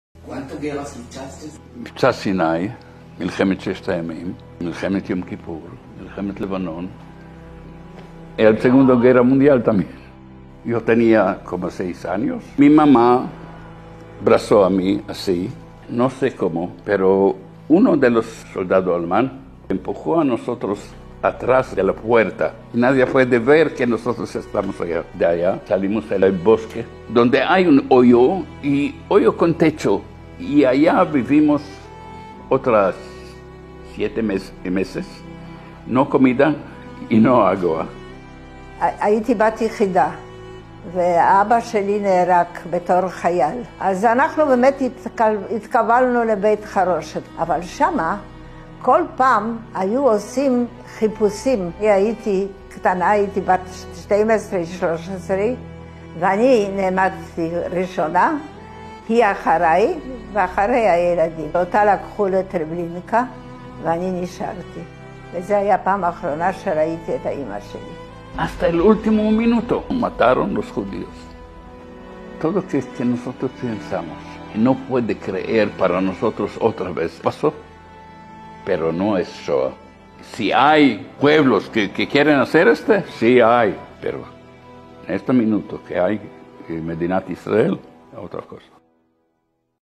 Lucharon y sobrevivieron
Ellos lucharon y sobrevivieron 5 guerras, incluyendo dos guerras mundiales. Y están aquí para contar que hoy más que nunca, vamos a vencer, y con Israel viviendo y el pueblo judío fuerte
Fuente: veo_israel